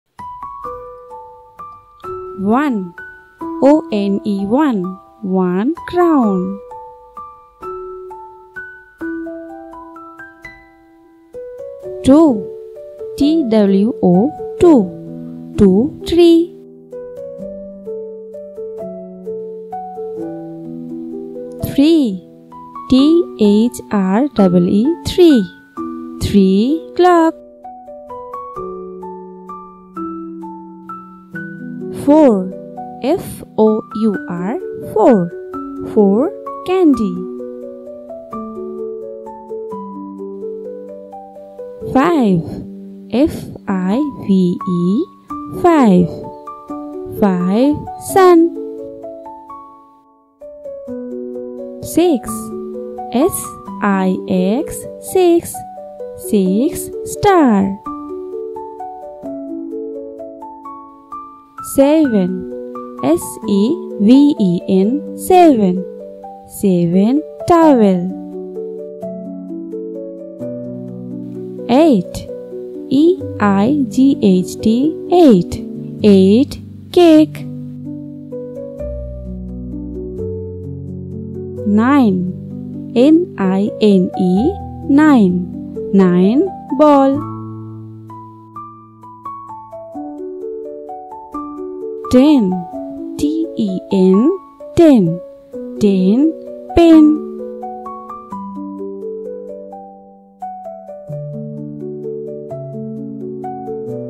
Counting Numbers 1 to 10 | Fun and Educational Video for Kids with Soothing Music
Welcome to our fun and educational video for kids! 🎉

In this video, children will learn to count numbers from 1 to 10. Each number is displayed on a slide with its spelling and a vibrant example picture to make learning engaging and enjoyable. The soothing background music helps create a calming and focused atmosphere, perfect for young learners.

Video Highlights:

Each number is paired with its spelling and an example picture
Relaxing and soothing background music
Why Kids Will Love This Video:

Bright and colorful visuals
Simple and clear presentation
Gentle and calming music
Perfect For:

Toddlers and preschoolers
Early learners
Parents and educators looking for quality educational content